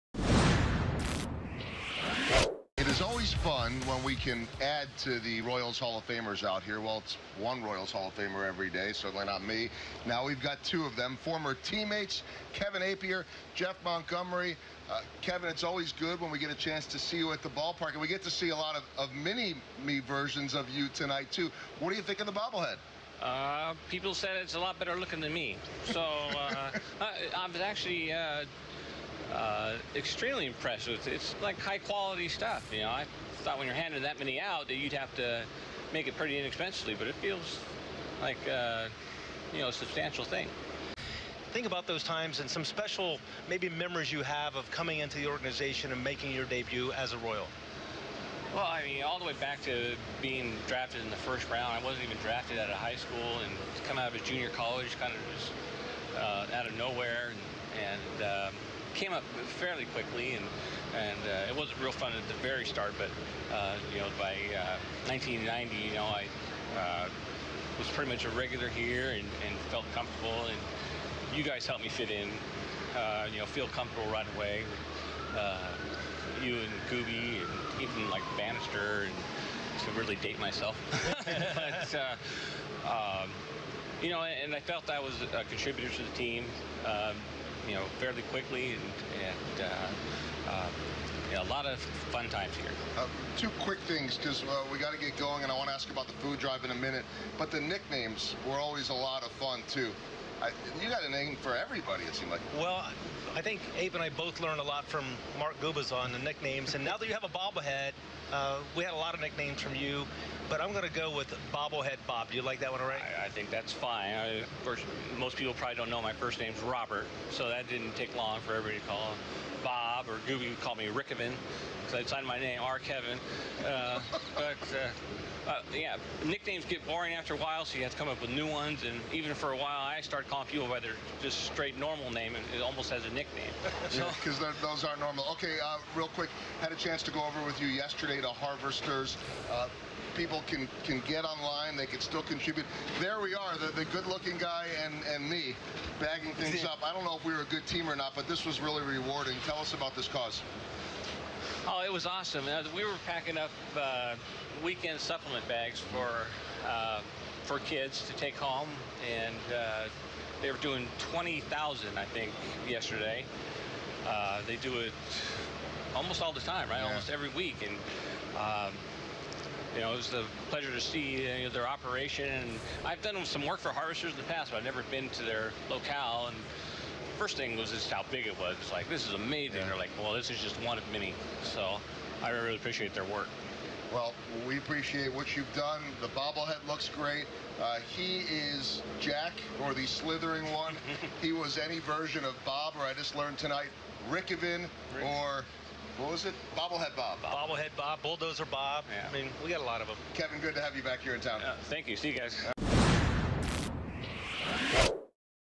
Kevin Appier on his bobblehead: "People said it's a lot better looking than me"
Royals Hall of Famer Kevin Appier shares the details on his many nicknames, which include Bob, Rkevin and his newest one, Bobblehead Bob.